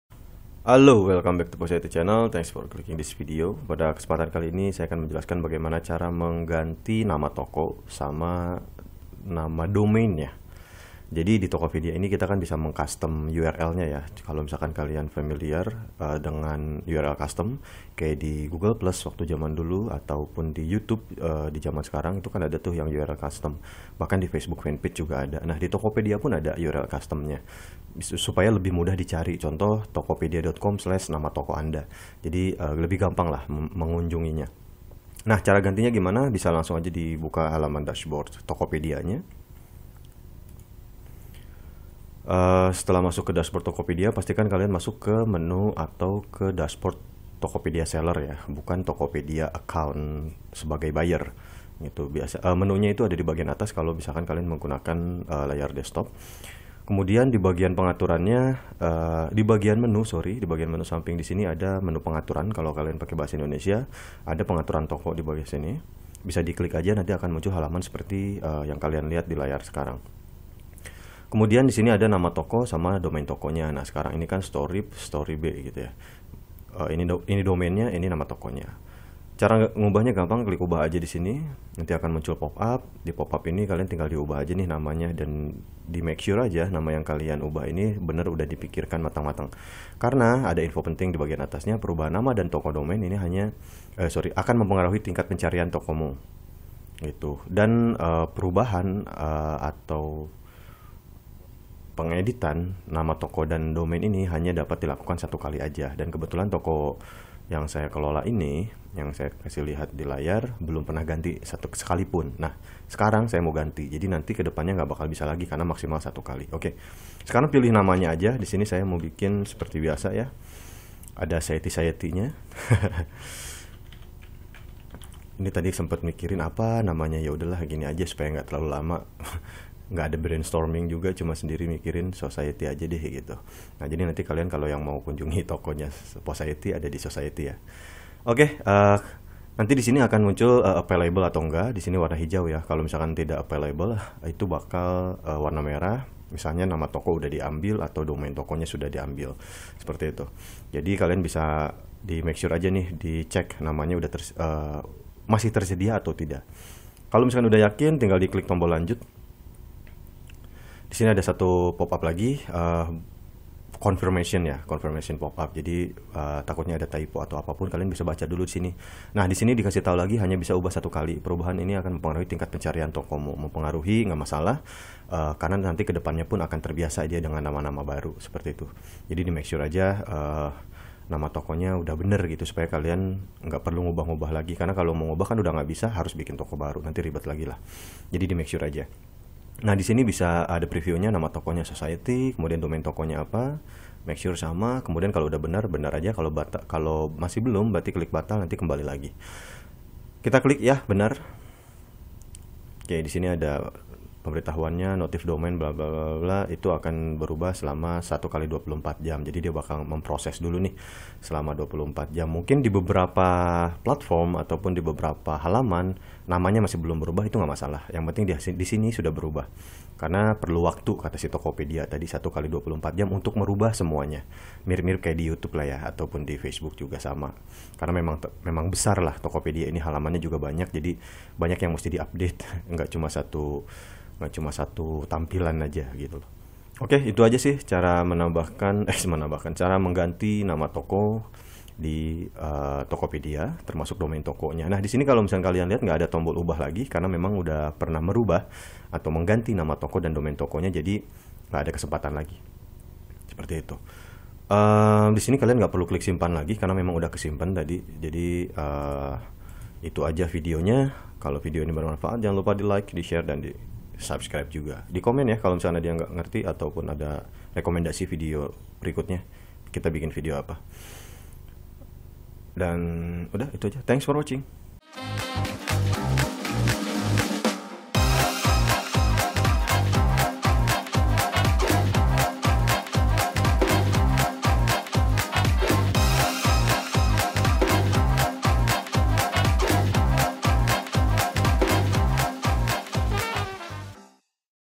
Cara Mengganti Nama Toko dan Domain Toko di TokoPedia
Tutorial Mudah Bagaimana Cara Ganti Nama Toko dan URL Custom Toko di TokoPedia.
Btw, mengganti nama toko dan URL kustom di TokoPedia hanya bisa dilakukan sekali. Video ini menjelaskan bagaimana cara mengubah nama toko dan domain toko di TokoPedia dengan benar, pastikan kamu sudah memiliki nama dan domain yang diinginkan sebelum...(Lanjutkan Membaca

DAFTAR PUTAR

atau

VIDEO SEBELUMNYA

Thanks! 😉